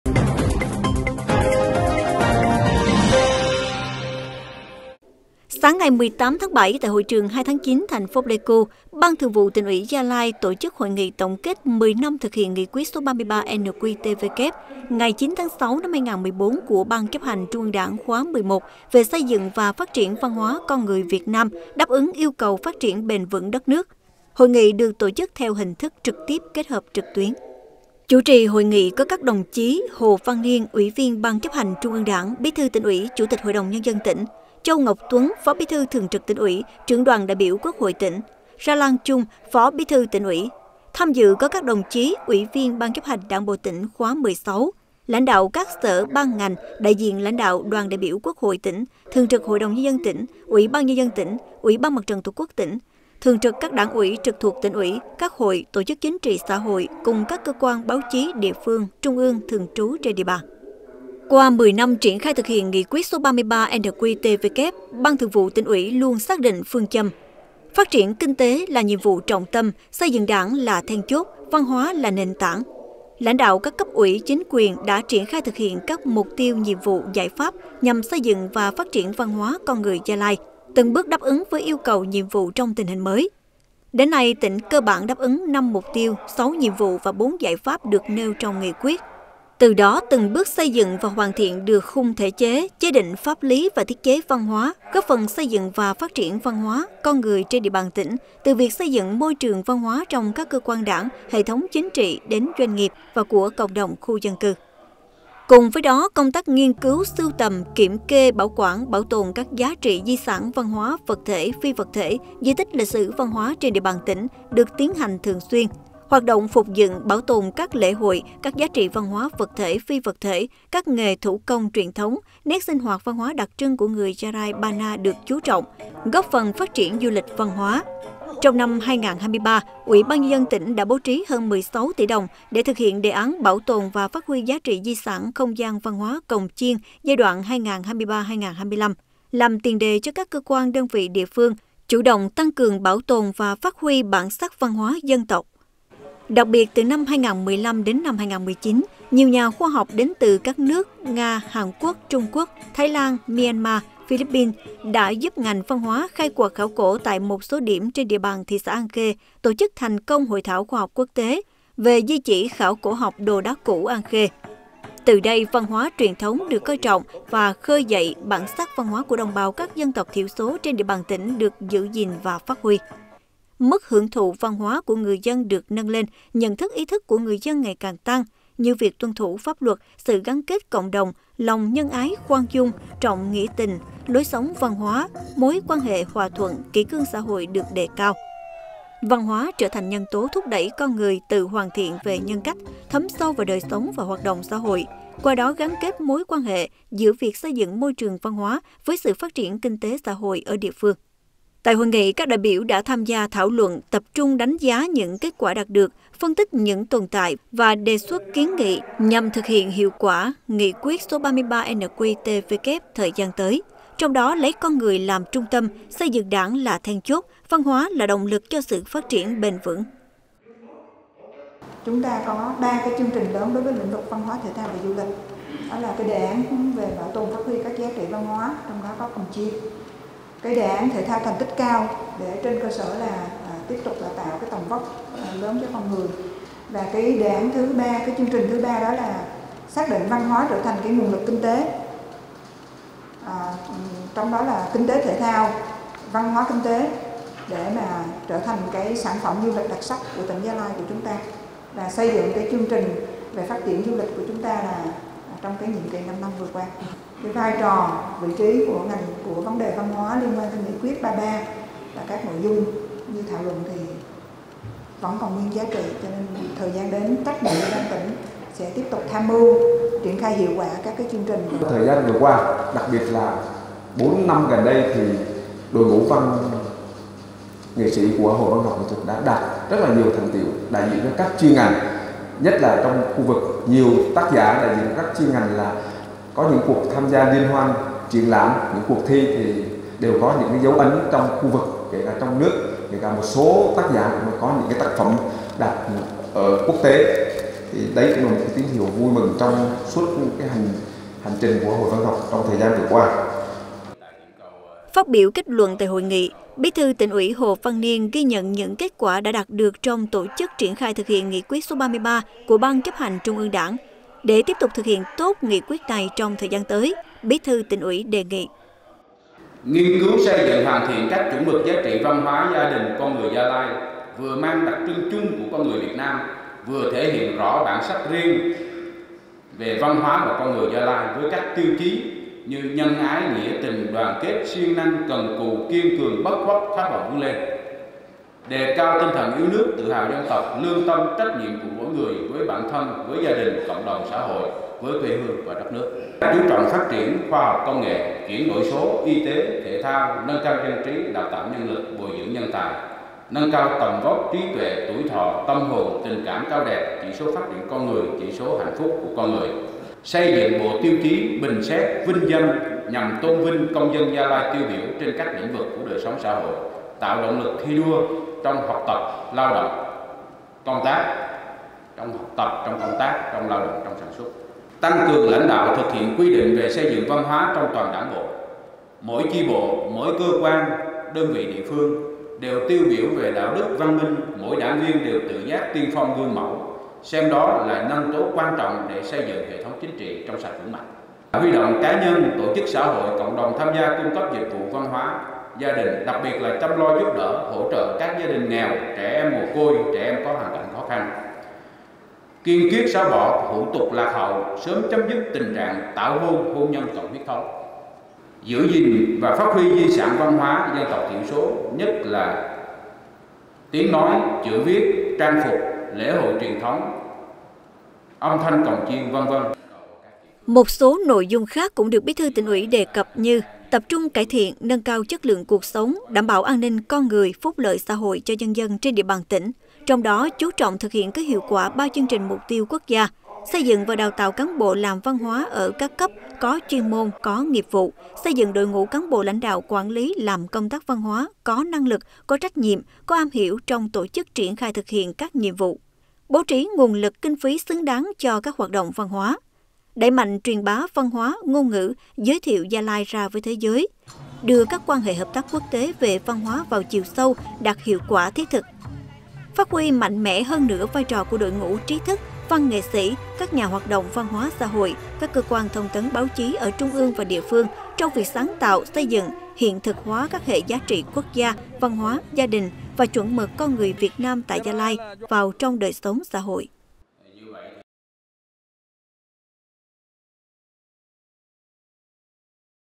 Hoàn thiện các chuẩn mực giá trị văn hóa, con người Gia Lai
Sáng 18-7,  Ban Thường vụ Tỉnh ủy Gia Lai tổ chức hội nghị tổng kết 10 năm thực hiện Nghị quyết số 33-NQ/TW, ngày 9-6-2014 của Ban Chấp hành Trung ương Đảng khóa XI về xây dựng và phát triển văn hóa, con người Việt Nam đáp ứng yêu cầu phát triển bền vững đất nước.

© Bản quyền thuộc về Báo Gia Lai
© Copyright by Báo Gia Lai and Partner☞ Do not Reup